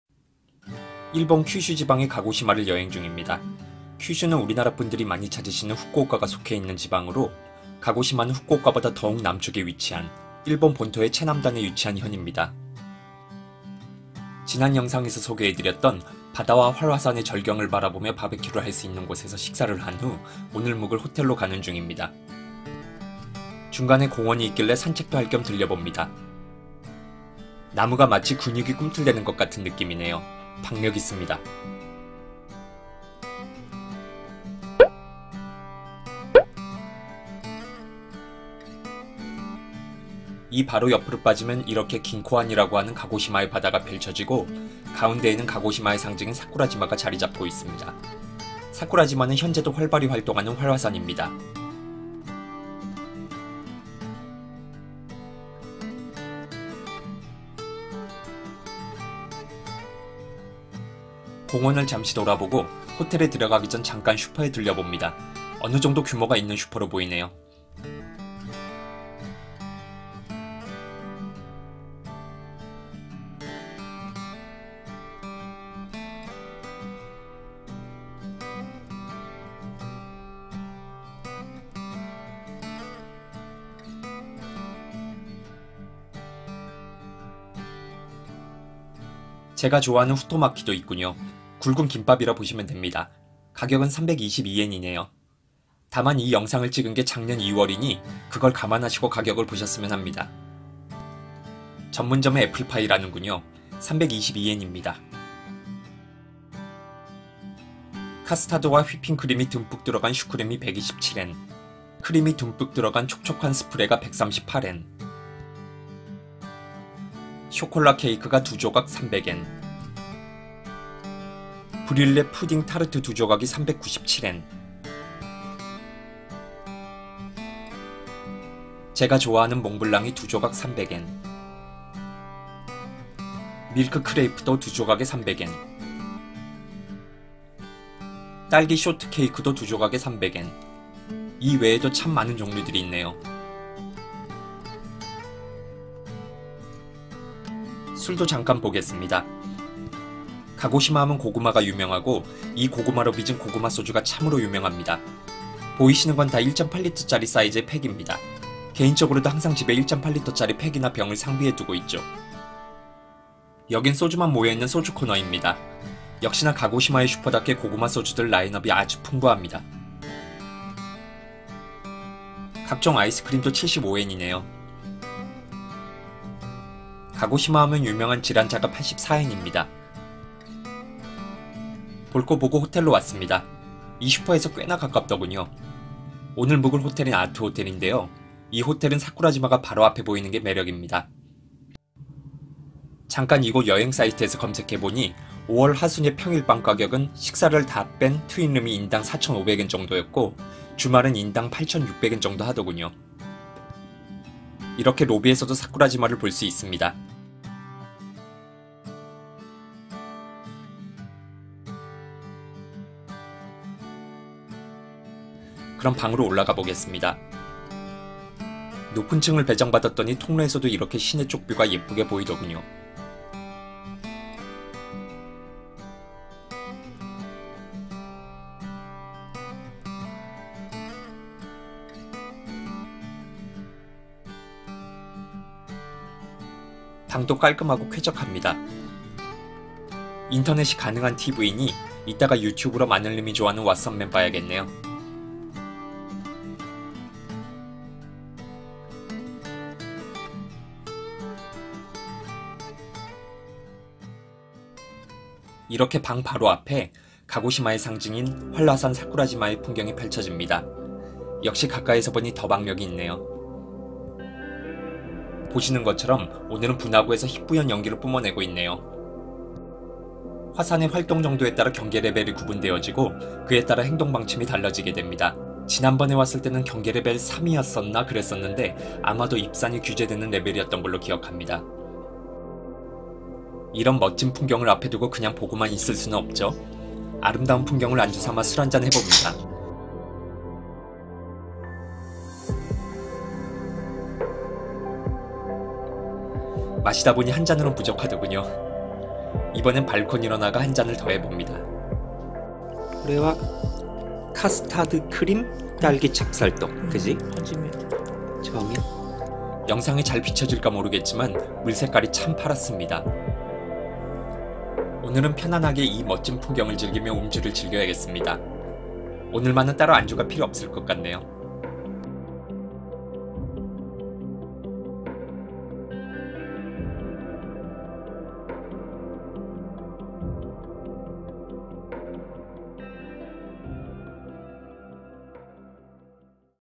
일본 마트 물가 l 규슈 가고시마 추천 호텔 정보 l 활화산 사쿠라지마가 한눈에 l 한일부부의 일본여행 브이로그 슈퍼마켓 정보 l ART 아트호텔
한일부부의 일본여행 브이로그, 일본 규슈 가고시마 ep.2입니다. 이번엔 방에서 가고시마의 상징인 활화산인 사쿠라지마가 정면으로 보이는 좋은 뷰를 가진 호텔을 소개해드릴까 합니다.

일본(日本) 큐슈지방의 가고시마(鹿児島)를 여행중입니다.
규슈(九州)는 우리나라 분들이 많이 찾으시는 후쿠오카가 속해있는 지방으로 가고시마는 후쿠오카보다 더욱 남쪽에 위치한, 일본 본토의 최남단에 위치한 현입니다.
지난 영상에서 소개해 드렸던 바다와 활화산의 절경을 바라보며 바베큐를 할 수 있는 곳에서 식사를 한 후 오늘 묵을 호텔로 가려합니다.

호텔로 들어가기 전, 잠깐 근처에 있는 마트에 들려보겠습니다.
보아하니 꽤 규모가 있는 슈퍼마켓으로 보이는군요.
일본의 물가는 어떤지 아주 살짝 살펴보고 가겠습니다.

오늘 소개해드릴 호텔은 아트호텔(アートホテル鹿児島)입니다.
이 호텔은 사쿠라지마가 바로 앞에 보이는 게 매력이지요.
잠깐 일본의 한 여행사이트에서 검색해보니 2023년 5월 하순의 평일 방 가격은 식사가 포함되지 않은 트윈룸이 인당 4500엔 정도였고, 주말은 인당 8600엔 정도 하더군요.

호텔의 방 안에선 박력있는 사쿠라지마의 모습을 볼 수 있습니다.
또한 사쿠라지마(桜島)와 더불어 긴코완(錦江湾)이라 불리는 가고시마의 바다도 함께 펼쳐지지요.
바닷물도 참으로 맑습니다.

이런 멋진 풍경을 앞에두고 그냥 보고만 있을 수는 없겠죠?
오늘은 편안하게 이 멋진 풍경을 즐기며 음주를 즐겨야겠습니다.ㅎㅎ
오늘만은 따로 안주가 필요 없을 것 같네요.

일본 자유여행이 재개되어 많은 분들이 일본으로 여행을 오고 계신데요, 이곳 가고시마는 우리나라에서도 가까운 규슈지역에 속해있다보니 왕래하기 좀 더 편한 곳이라는 생각이 드네요.
유명 관광지와는 조금 다른, 이곳만의 분위기와 매력을 느끼고 싶으신 분들에게 조금이라도 이 영상이 도움이 되었으면 합니다.